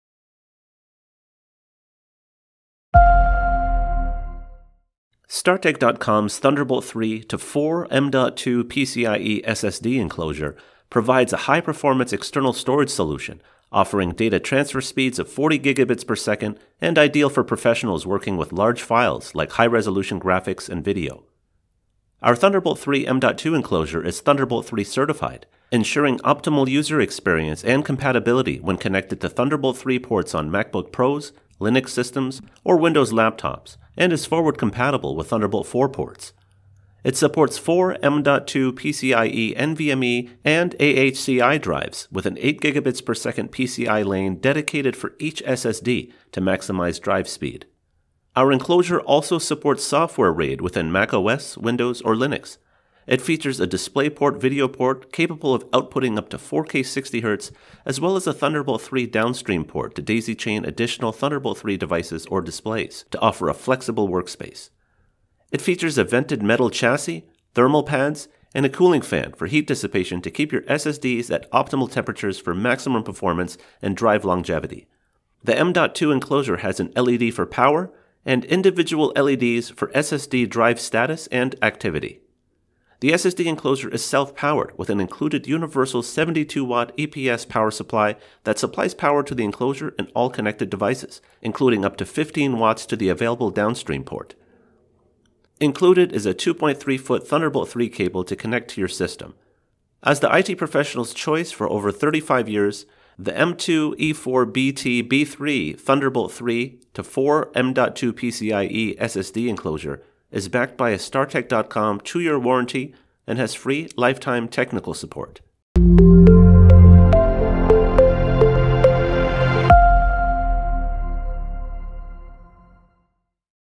4-Bay Thunderbolt 3 Enclosure for M.2 NVMe | StarTech.com
The Thunderbolt 3 M.2 NVMe enclosure turns 4x M.2 NVMe SSDs into a high-performance external storage solution, supporting data transfer speeds of up to 40Gbps. It is ideal for professionals working with large files, such as high-resolution graphics or videos. The TB3 SSD Enclosure is Thunderbolt 3 certified and features a downstream Thunderbolt 3 port to connect additional Thunderbolt 3 devices. A DisplayPort output allows you to connect an external 4K 60Hz display. It also includes a cooling fan for drive longevity and reliable performance.

Product Features:
·        Thunderbolt 3 Certified
·        4K 60hz DisplayPort Output
·        Downstream Thunderbolt 3 Port to Daisy Chain Additional Devices
·        Includes a 2ft Thunderbolt 3 Host Cable

THE IT PRO'S CHOICE: Designed and built for IT Professionals, this external drive enclosure is backed for 2-years, including free lifetime 24/5 multi-lingual technical assistance.